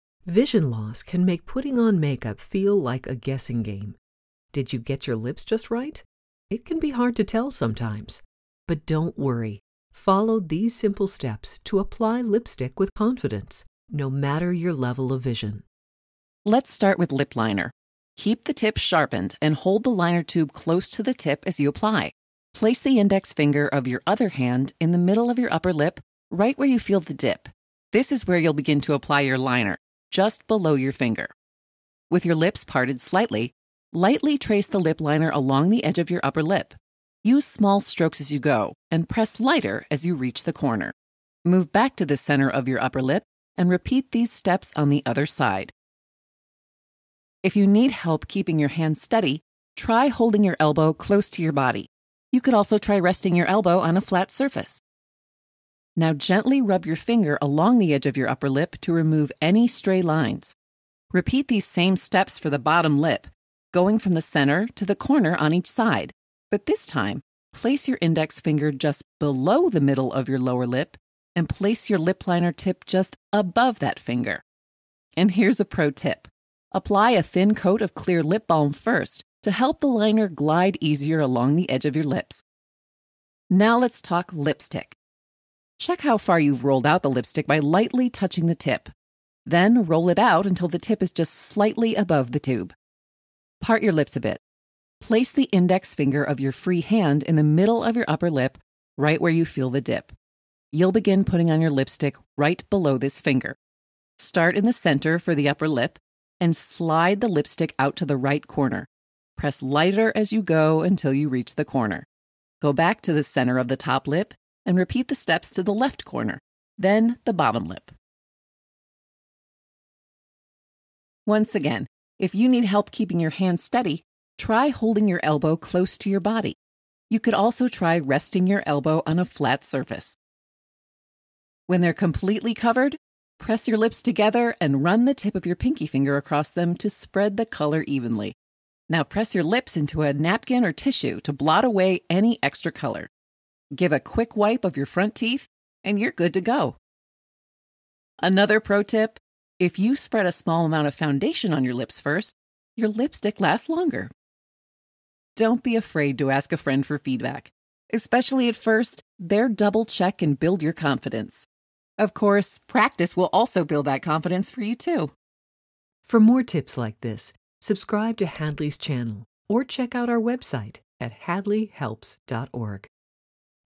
HOW TO APPLY LIPSTICK WITH LOW VISION
Learn how to apply lipstick with low vision! This step-by-step tutorial offers practical tips and techniques for anyone with a visual impairment. Discover how to enhance your beauty routine with confidence, no matter your vision.